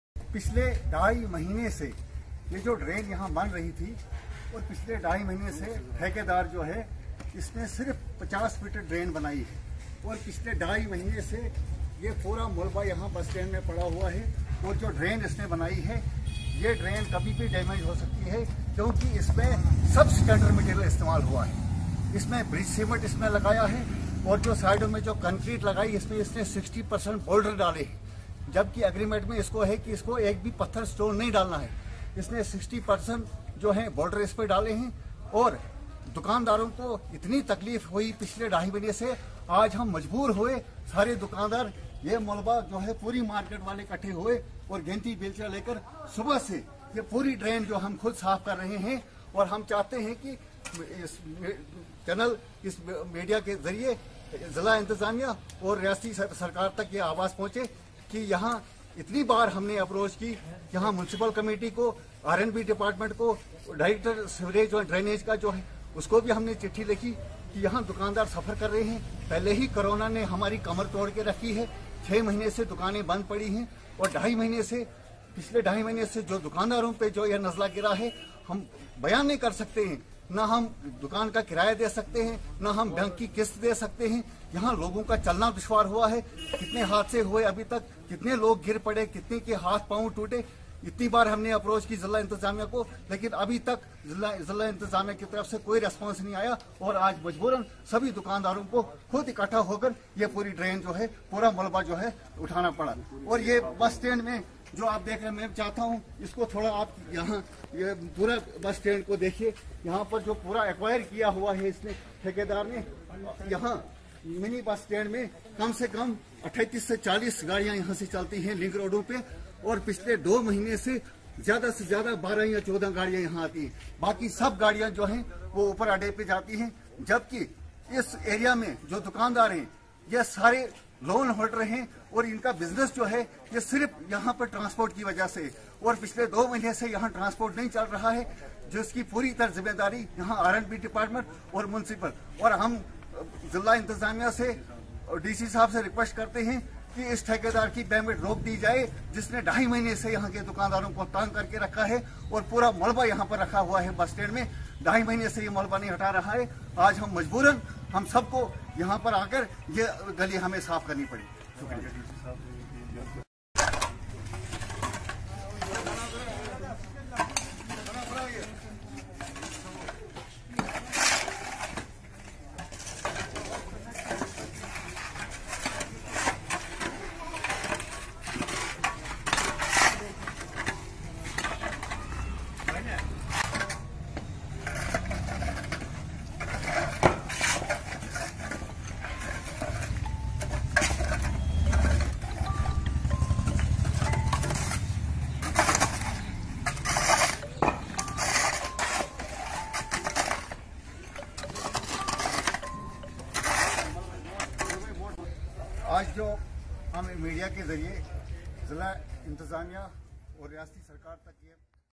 District President Youth National Conference D D Farooq demands enquiry in substandard material used
District President Youth National Conference D.D Farooq demands enquiry in substandard material used in construction of drainage